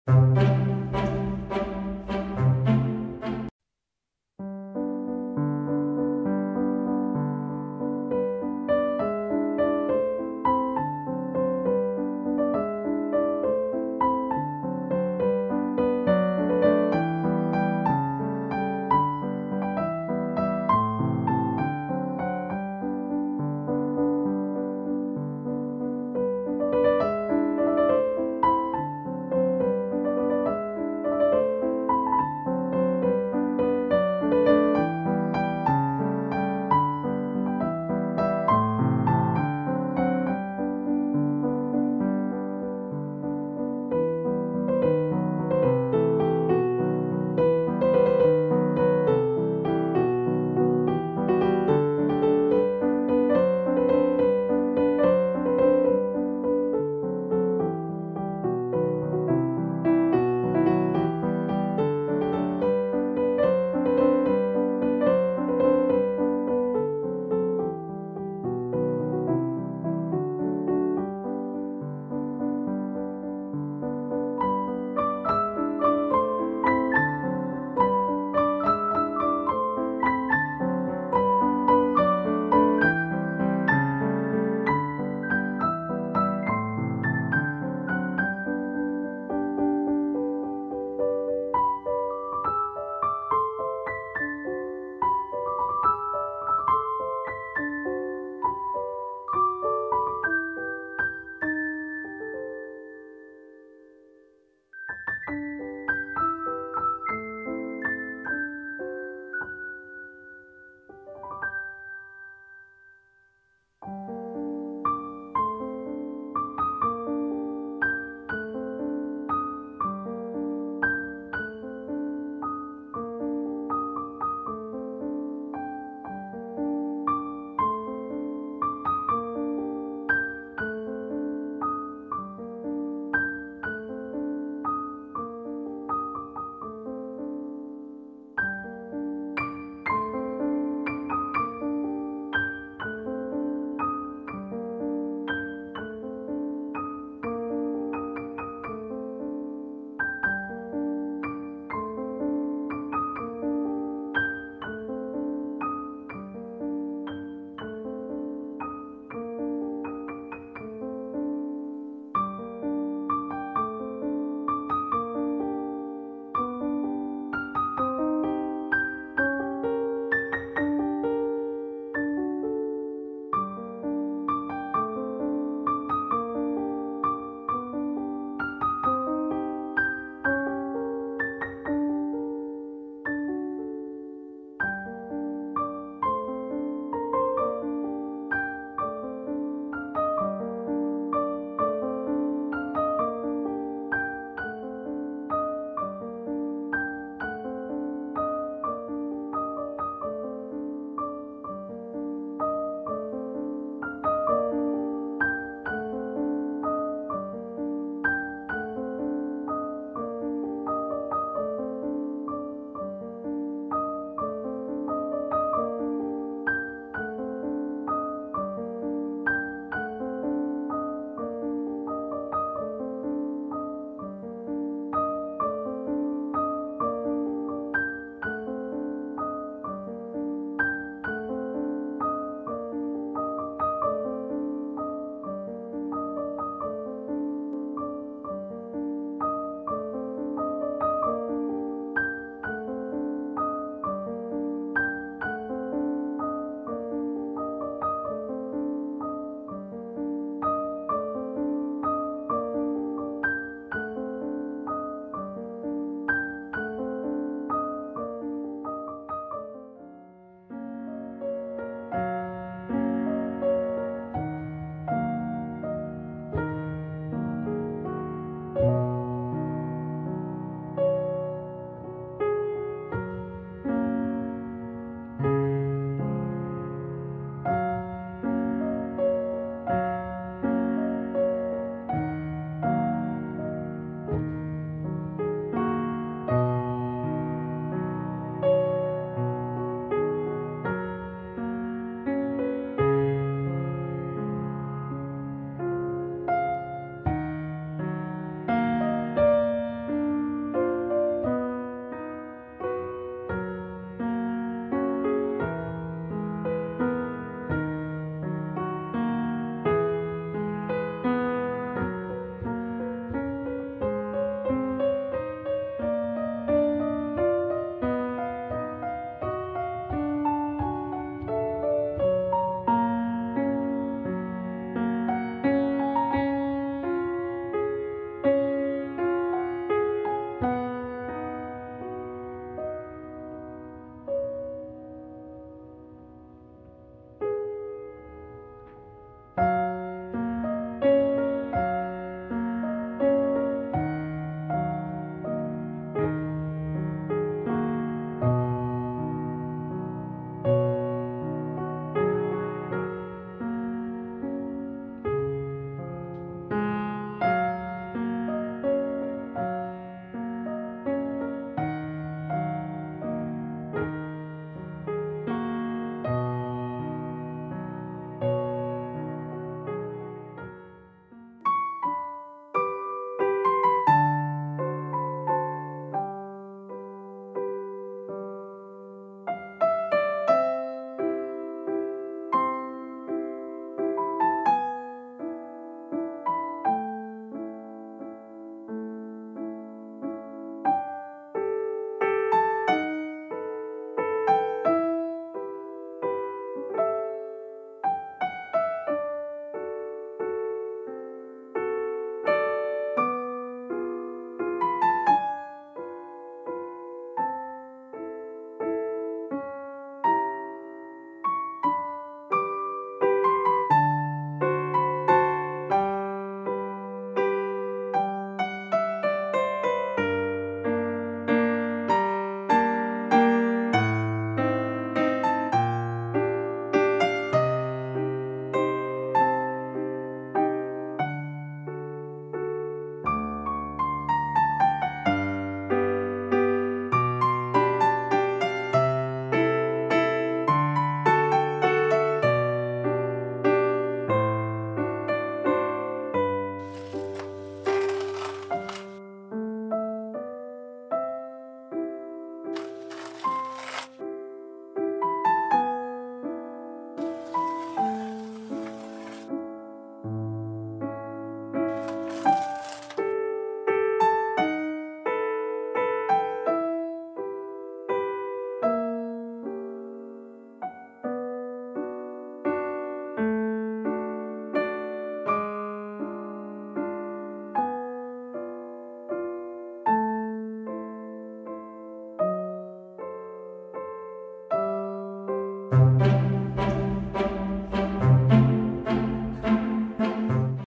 Sunset Аcrylic Painting | Flying Cranes | Art challenge #30
Easy acrylic landscape painting for beginners. How to paint flying cranes at sunset.

Canvas size: 30 x 30 cm